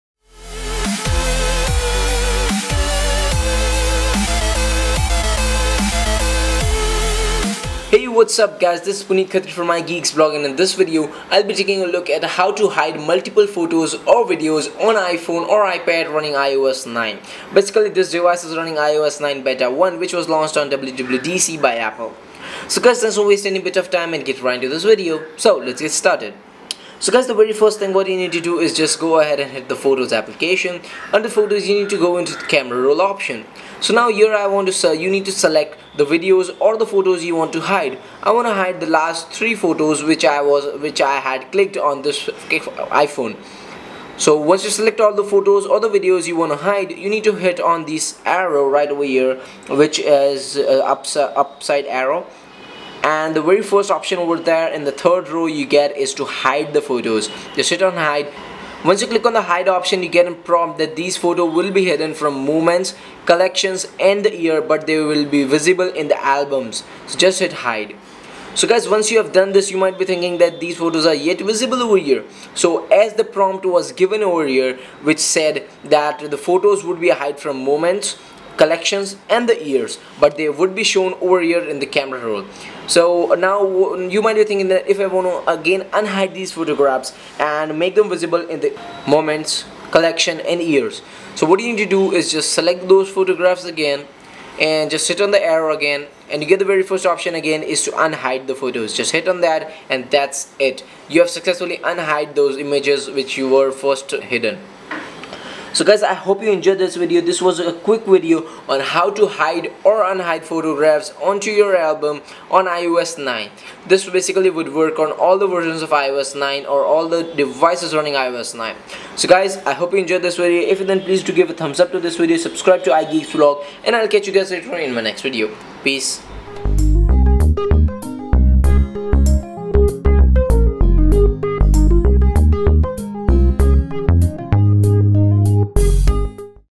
Hide Multiple Photos in iOS 9 on iPhone or iPad
In this video we walk you through how to hide multiple photos at once in iOS 9 on your iPhone or iPad.